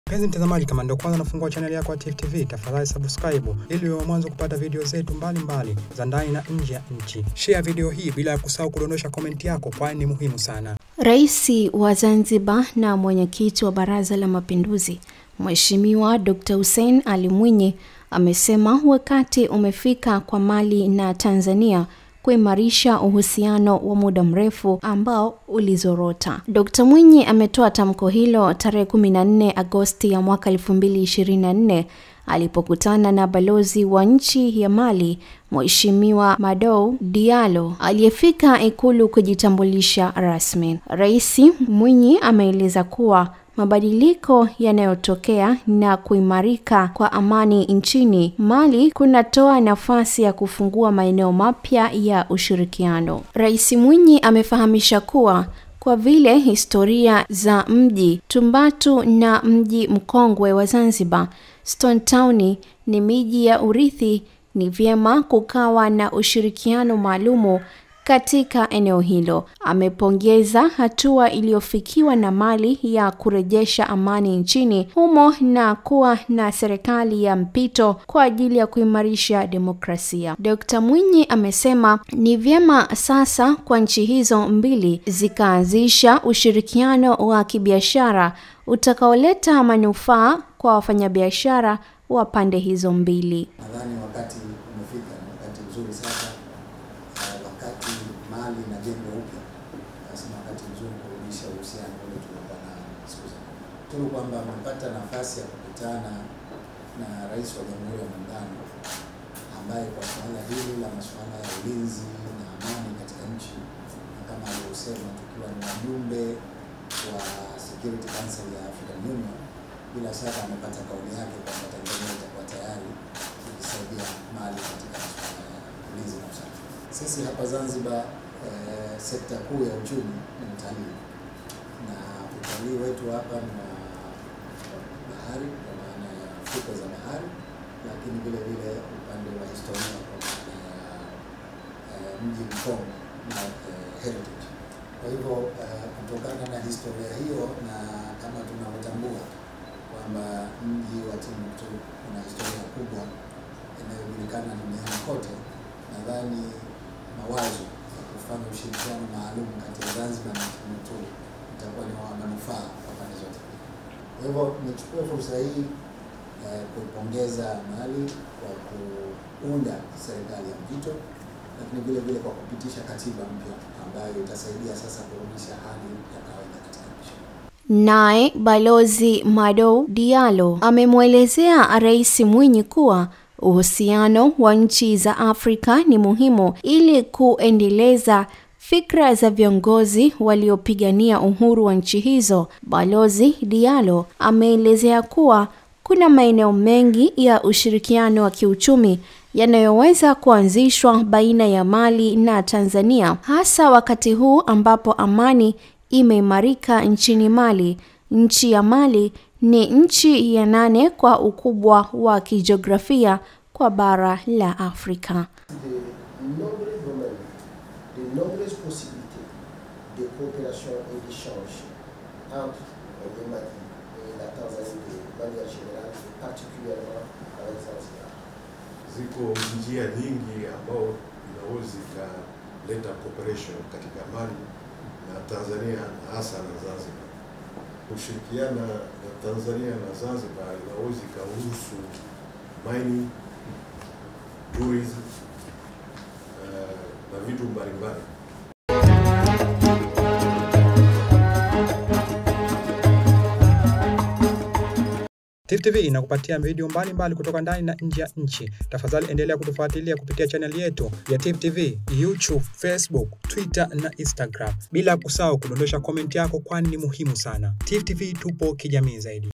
IKULU ZANZIBAR RAIS MWINYI AMANI NCHINI MALI NA SERIKALI YA MPITO KWA AJILI YA KUIMARISHA DEMOCRASIA
Rais wa Zanzibar na Mwenyekiti wa Baraza la Mapinduzi Dk, Hussein Ali Mwinyi amesema wakati umefika kwa Mali na Tanzania kuimarisha Uhusiano wa muda mrefu ambao ulizorota.

Dk, Mwinyi ametoa tamko hilo tar 14Agost 2024 alipokutana na Balozi wa nchi ya Mali Mh, Madou Diallo aliefika Ikulu kujitambulisha
Rasmi.

Rais Mwinyi ameeleza kuwa Mabadiliko yanayotokea na kuimarika kwa Amani nchini Mali kunatoa nafasi ya kufungua maeneo mapya ya ushirikiano.

Rais Mwinyi amefahamisha kuwa kwa vile historia za Mji Tumbuktu na mjini Mkongwe wa Zanzibar [stone town]ni miji ya Urithi ni vyema kukawa na ushirikiano maalum katika eneo hilo.

Amepongeza hatua iliofikiwa na Mali ya kurejesha Amani nchini humo na kuwa na Serikali ya Mpito kwa ajili ya kuimarisha demokrasia.

Aidha Dk, Mwinyi amesema ni vyema sasa kwa nchi hizo mbili zikaanzisha Ushrikiano wa kibiashara utakaoleta manufaa kwa Wafanyabiashara wa pande hizo mbili.

Nae Balozi Madou Diallo amemuelezea Rais Mwinyi kuwa Uhusiano wa Nchi za Afrika ni muhimu ili kuuendeleza fikra za Viongozi waliopigania Uhuru wa nchi hizo.
Balozi Diallo ameeleza kuwa kuna maeneo mengi ya Ushirikano wa kiuchumi yanayoweza kuanzishwa baina ya Mali na Tanzania hasa wakati huu ambapo Amani imeimarika nchini Mali. Nchi ya Mali ni nchi ya nane kwa ukubwa wa kijeografia kwa Bara la Afrika.